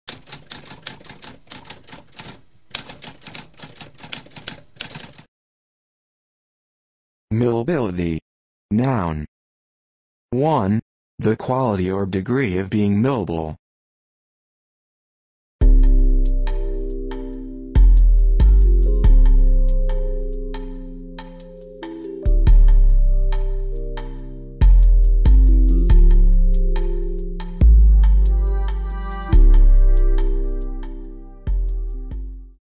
What does millability mean?
A spoken definition of millability.

Intro Sound:
Typewriter - Tamskp